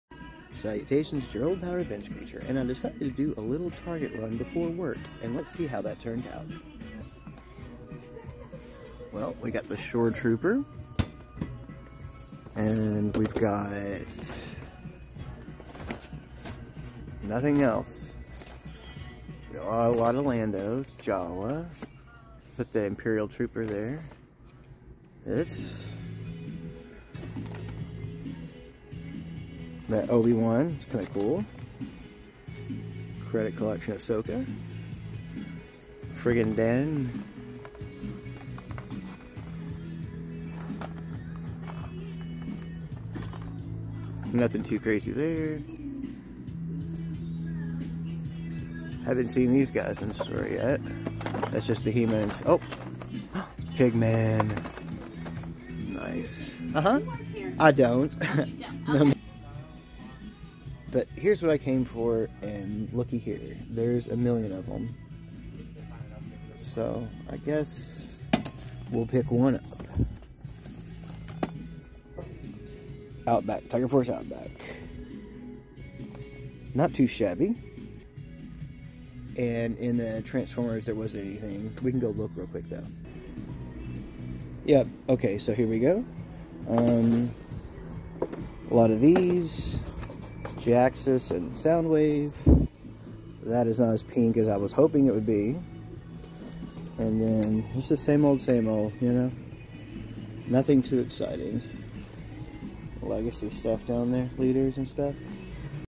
Revenge Creature: Target Run! 12/14/22
Salutations, gang! It's your old pal Revenge Creature and I'm back with another low effort video! I happened to have some time to kill before going to work so I decided to hit Target and see what goodies I might find! Did it pay off? Watch and see! OSQUINOXO!!